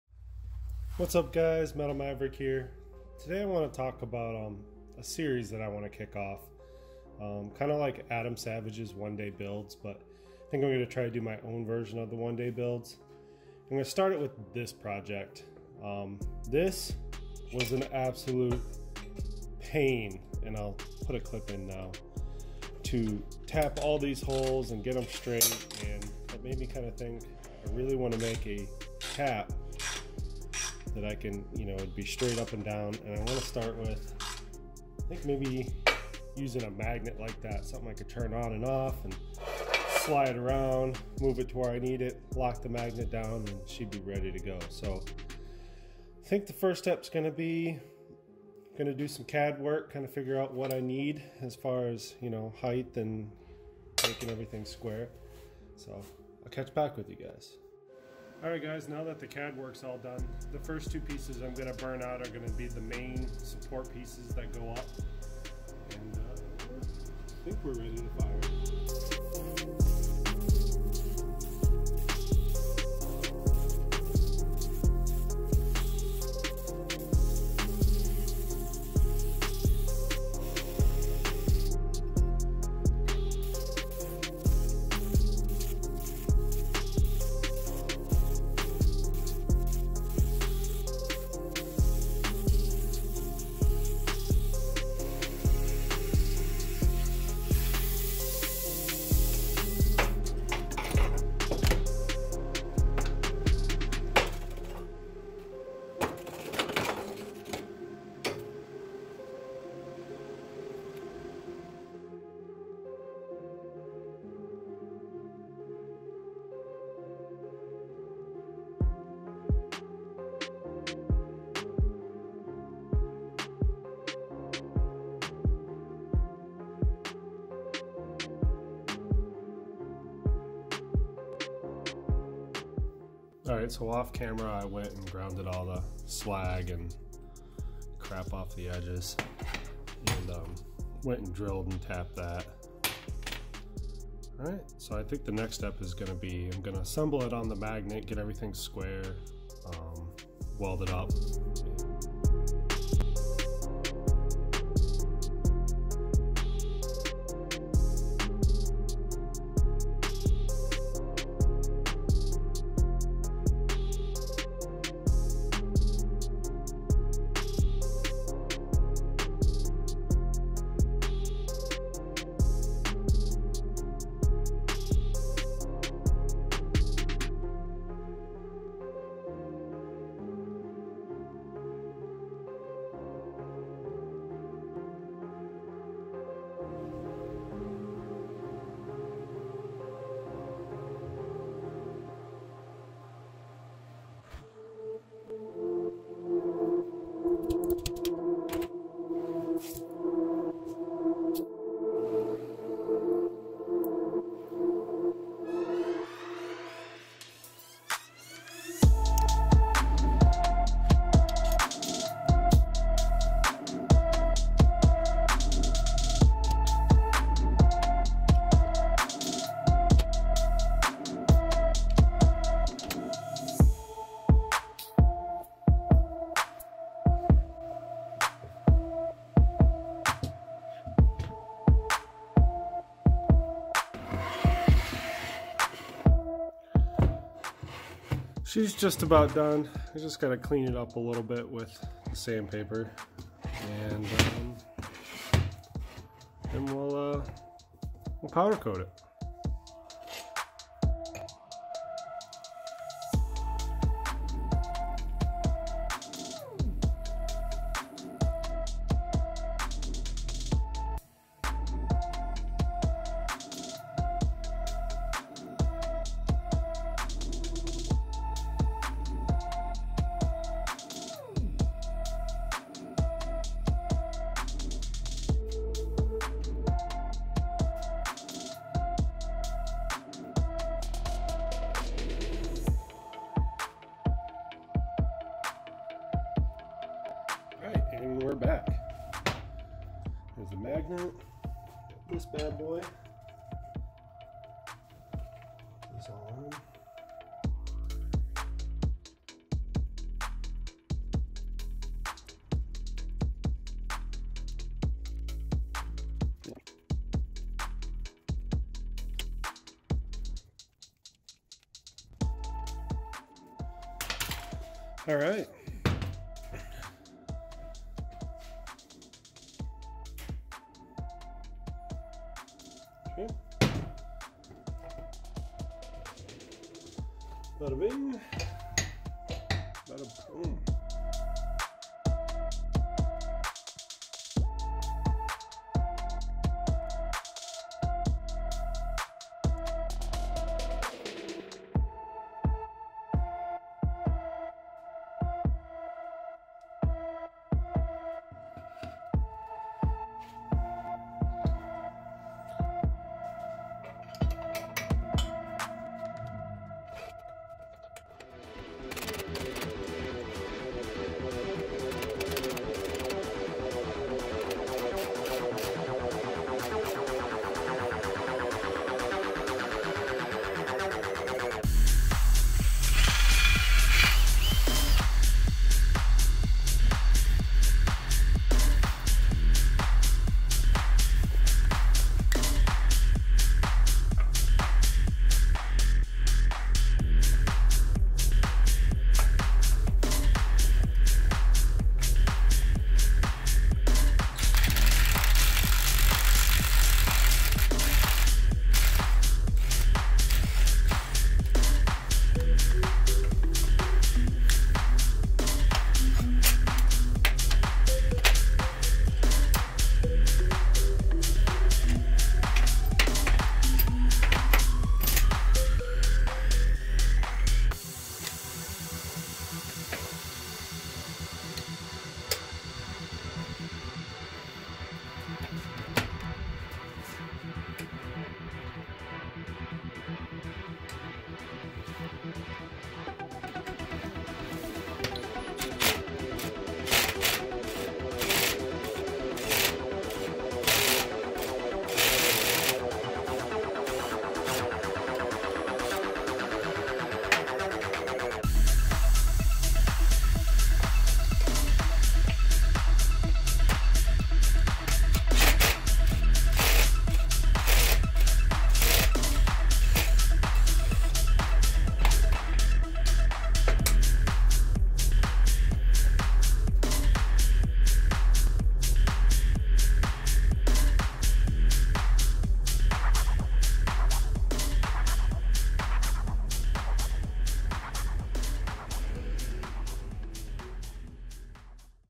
Magnetic taping fixture tool
I decide to make my life easier after all the hard work was done, to make a magnetic taping tool that holds your tap perpendicular to your work piece so you get perfectly tapped holes every time.

This is the first video in a new series I'm starting called One day builds where hopefully I can crank theses cool projects out in a Afternoon.